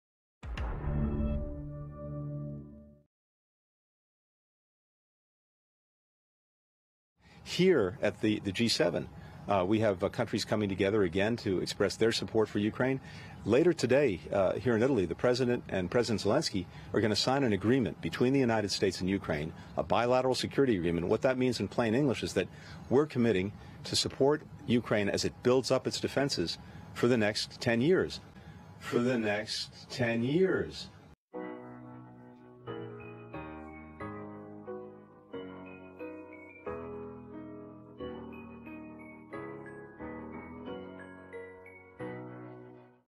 Biden and Zelensky to sign deal to "SUPPORT" Ukraine for 10 MORE YEARS
Bruh.

ADDITIONAL CREDITS
➡️Memology 101 Netflix Intro:

Trends